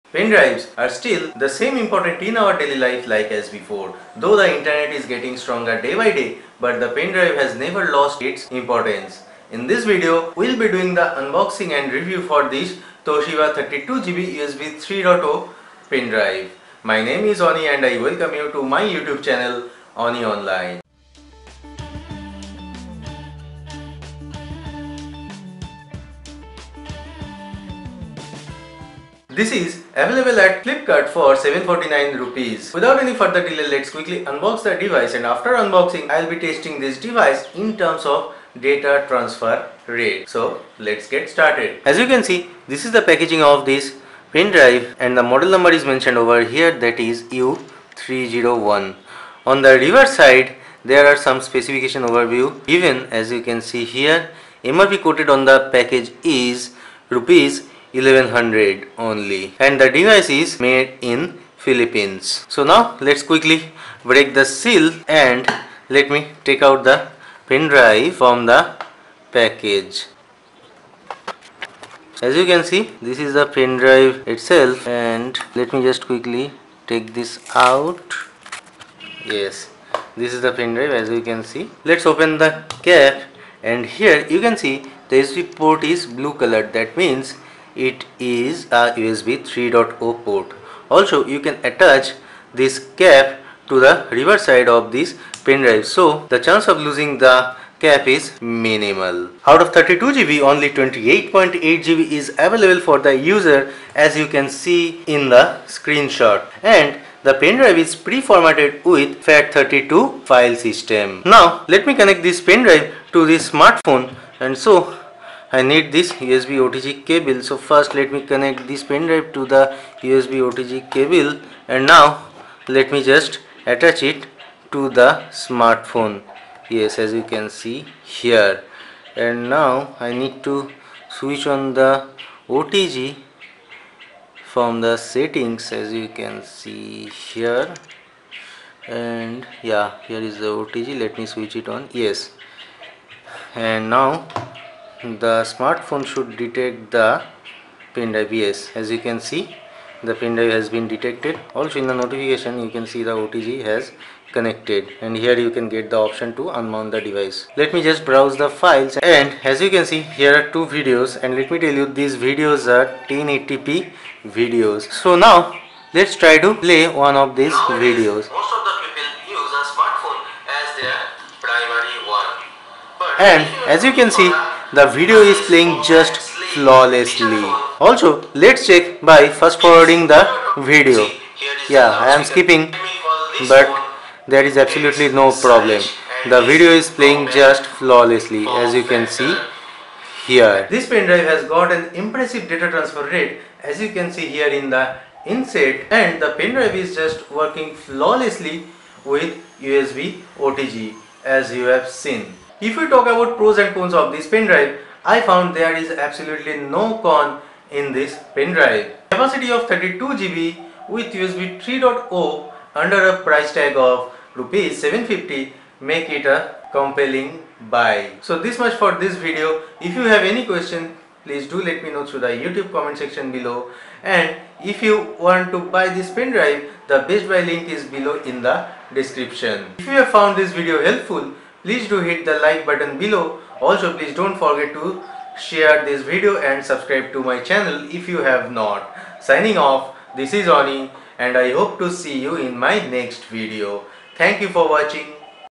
Toshiba U310 (32GB, USB 3.0) Pen Drive- Unboxing and Review | Speed Test Results
Please watch this video till the end for detailed review with speed test results.